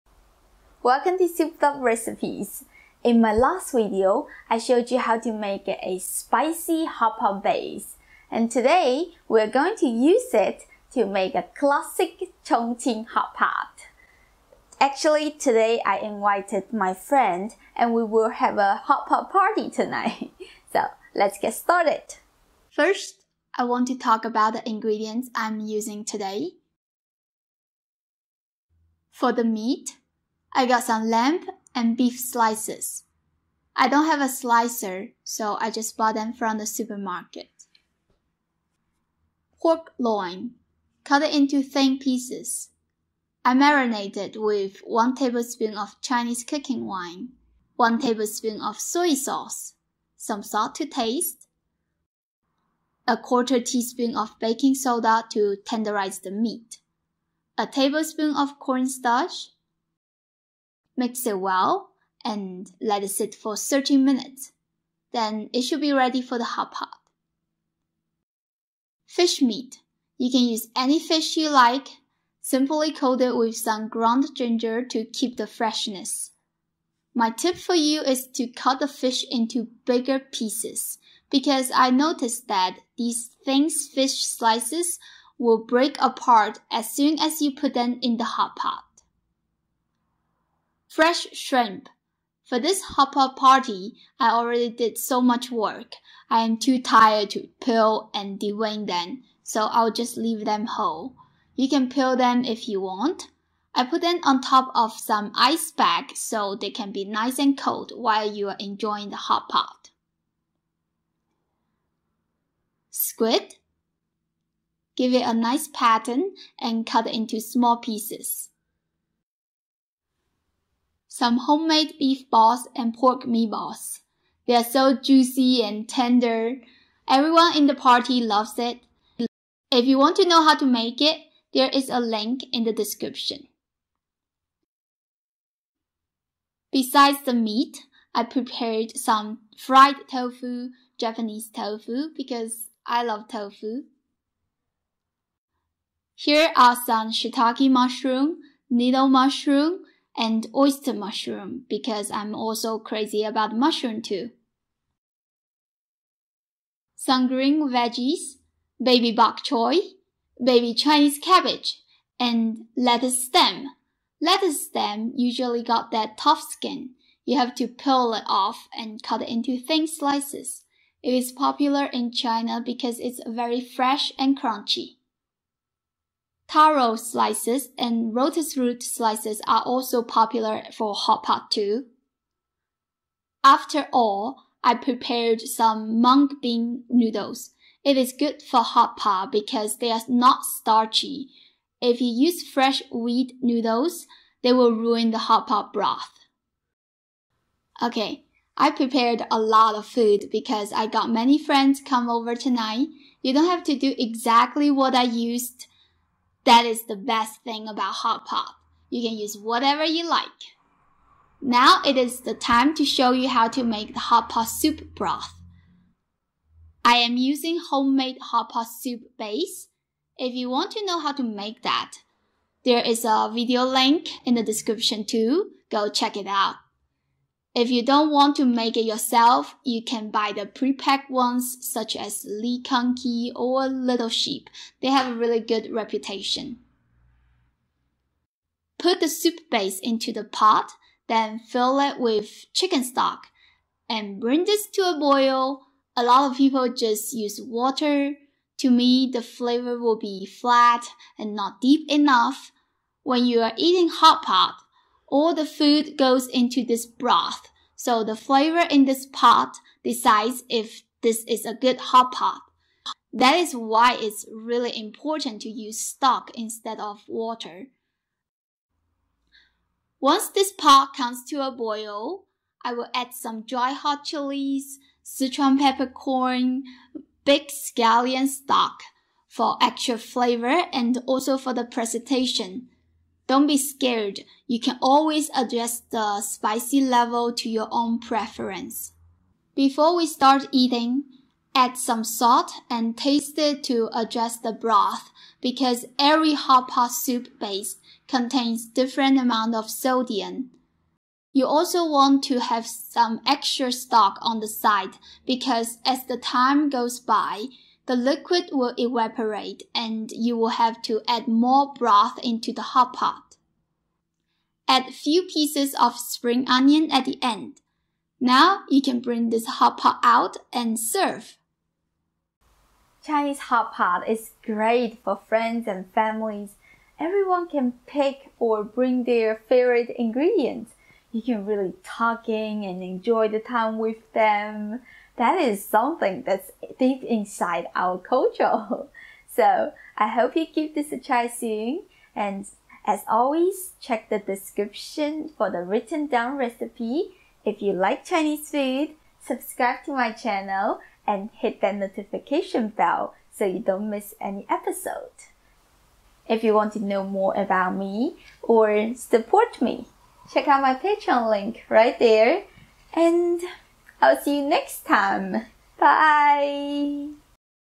Homemade Spicy and Numbing Hot Pot Recipe [麻辣火锅]
🔪PURCHASE A KNIFE SET

Chinese hot pot is great for friends and family. Everyone can pick or bring their favorite ingredients. You can really tuck in and enjoy the time with them. That is something deep inside our culture. In my last video, I show you how to make a spicy hot pot base and today we are going to use it to make a classic Chong Qing spicy numbing hot pot.

**Ingredients for the Side**

- Beef slices [肥牛片]
- lamb slices [肥羊片]
- marinated pork [调味猪肉片]
- Fish slices [生鱼片]
- Peeled shrimp [鲜虾]
- squid [鱿鱼]
- beef balls [牛肉丸]
- pork balls [猪肉丸]
- fried tofu [油豆腐]
- Japanese tofu [日本豆腐]
- shiitake mushroom [香菇]
- noddle mushroom [金针菇]
- Oyster mushroom [平菇]
- Baby Chinese cabbage [娃娃菜]
- Spinach [菠菜]
- lettuce [生菜]
- lettuce stem [莴笋]
- taro [芋头]
- lotus root [藕片]

**Ingredients for the Soup Base**

- 2 cups of homemade soup base [自制火锅底料]
- 8 cups of chicken stock [鸡肉高汤]
- 1.5 cups of hot dried chilies [干红辣椒] [Amazon
- 1/4 cups of Sichuan peppercorn [花椒] [Amazon
- Few stalks of big scallion [大葱]
- Few pieces of spring onion [小葱]

**Instructions **

- Hotpot is very easy and flexible. Just prepare the sides that you want to eat with the hot pot. Then make the hot pot soup.

- Put the soup base into the pot then fill it with chicken stock. Bring the pot to a boil. Add the dried hot chilies, Sichuan peppercorn, big scallion stalk for extra flavor and also for the presentation.

- Before we started eating, add some salt and taste it to adjust the broth because every hot pot soup base contains different amount of sodium.

- You also want to have some extra stock on the side because as the time goes by, the liquid will evaporate and you will have to add more broth to the pot.

- Add few pieces of spring onion at the end. Now you can bring this pot out and serve.

Enjoy! If you have any questions about the recipes, just post a comment, will help you out as soon as possible!

RELATED RECIPES -

You can also buy pre-packed hot pot soup base from Amazon